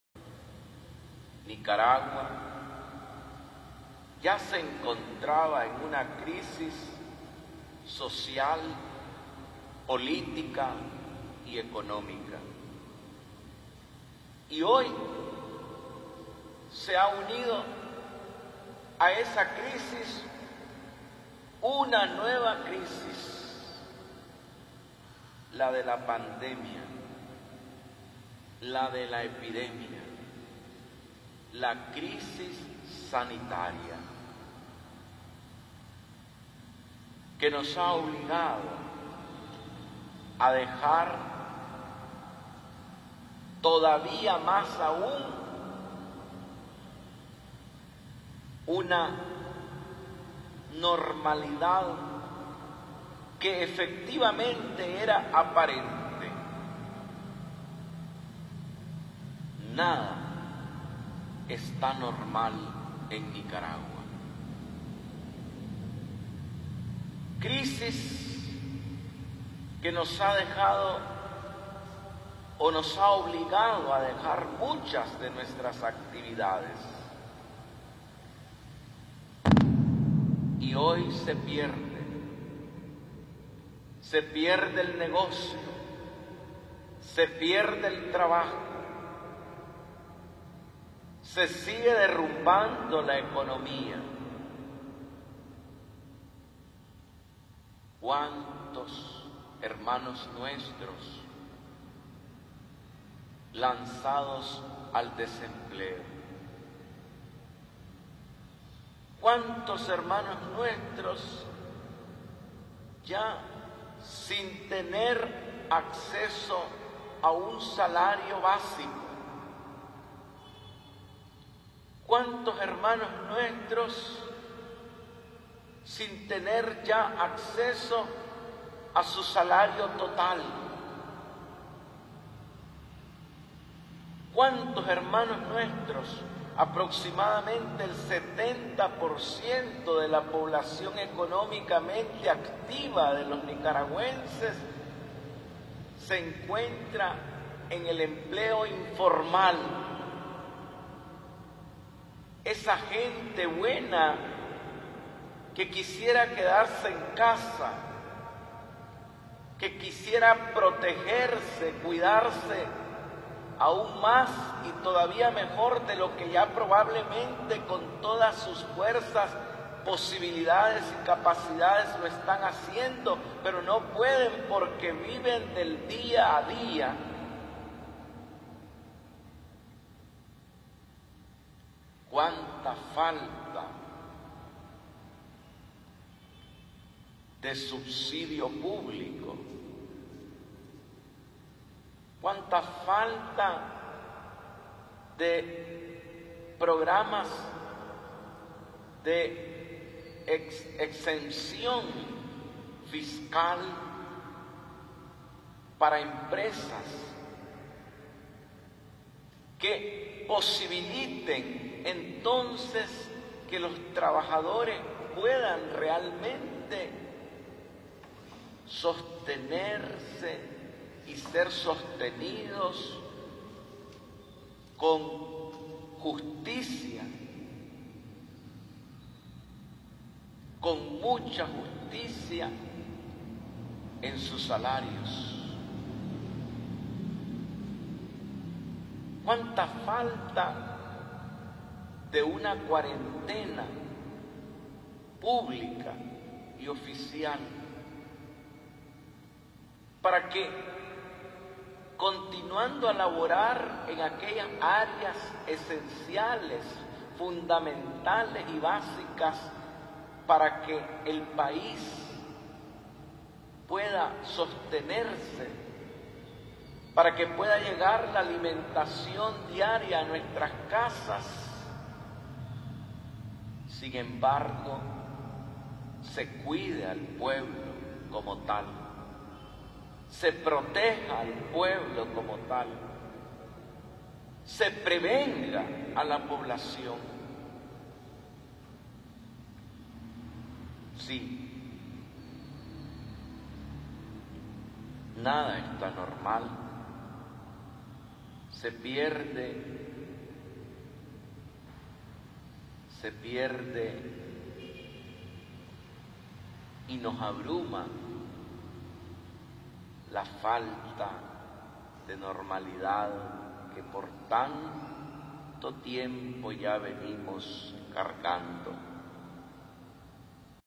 Mons. Rolando Álvarez "Nada está normal en Nicaragua"
"Nada está Normal en Nicaragua", recuerda monseñor Rolando Alvarez, el domingo 26 de abri, al referirse a la doble crisis que enfrentan el pueblo de Nicaragua.